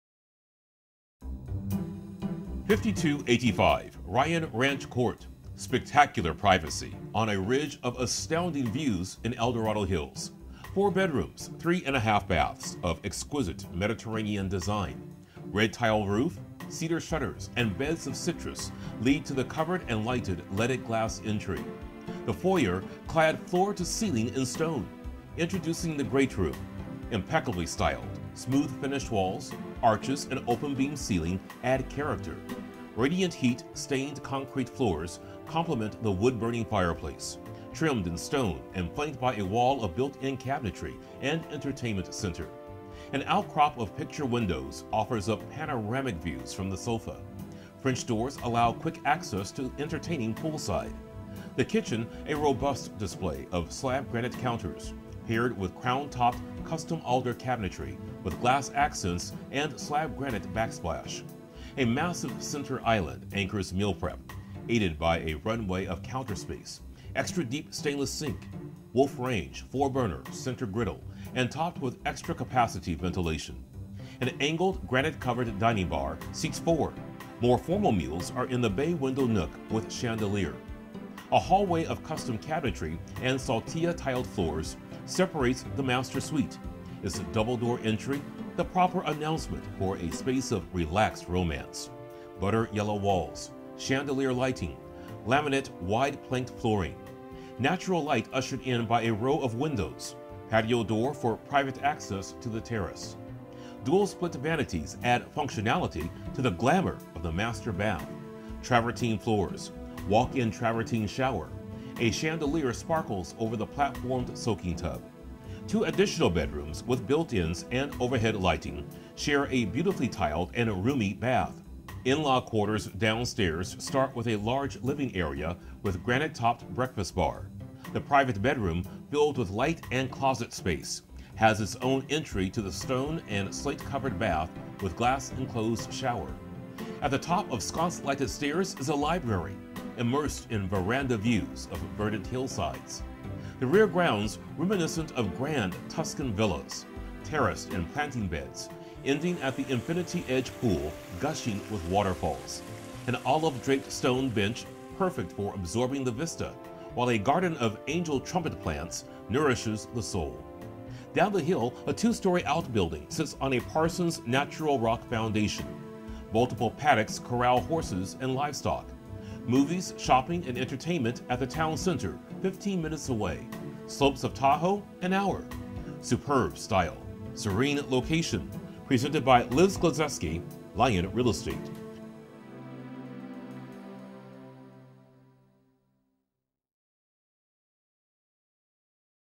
Home For Sale! 5285 Ryan Ranch Ct., El Dorado Hills, CA
5285 Ryan Ranch Court. Spectacular privacy on a ridge of astounding views in El Dorado Hills. Four bedrooms, three and half baths of exquisite Mediterranean design- red tile roof, cedar shutters and beds of citrus lead to the covered and lighted leaded glass entry.  The foyer, clad floor to ceiling in stone, introduces the great room. Impeccably styled- smooth finished walls,  arches and open beam ceiling, add character.  Radiant heat, stained, concrete floors complement the wood burning fireplace, trimmed in stone and flanked by a wall of built-in cabinetry and entertainment center. An outcrop of picture windows offers up panoramic views from the sofa.  French doors allow quick access to entertaining poolside.  The kitchen, a robust display of slab granite counters paired with crown topped, custom alder cabinetry, with glass accents and slab granite backsplash.  A massive center island anchors meal prep, aided by a runway of counter space, extra deep stainless sink, Wolf range four burner center griddle, and topped with extra-capacity ventilation. An angled granite covered dining bar seats four, more formal meals are in the bay window nook with chandelier. A hallway of custom cabinetry and saltilla tiled floors separates the master suite. Its double door entry the proper announcement for a space of relaxed romance. Butter yellow walls, chandelier lighting, laminate wide planked flooring.  Natural light ushered in by a row of windows- patio door for private access to the terrace.  Dual split vanities add functionality to the glamour of the master bath. Travertine floors, walk in travertine shower, a chandelier sparkles over the platformed soaking tub.  Two additional bedrooms with built-ins and overhead lighting share a beautifully tiled, and roomy bath.  In-law quarters downstairs start with a large living area with granite topped breakfast bar.  The private bedroom filled with light and closet space, has its own entry to the stone  and slate covered bath with glass enclosed shower.  At the top of sconce lighted stairs is a library immersed  in  verandah views of verdant hillsides.   The rear grounds, reminiscent of grand Tuscan villas, terraced in planting beds, ending at the infinity edge pool, gushing with waterfalls.  An olive draped stone bench is perfect for absorbing the vista, while a garden of angel trumpet plants nourishes the soul.       Down the hill a two story out building sits on a Parsons natural rock foundation.  Multiple paddocks corral horses and livestock. Movies shopping, and entertainment at the town center 15 minutes away. Slopes of Tahoe, an hour.  Superb style, serene location.  Presented by Liz Glezewski, Lyon Real Estate.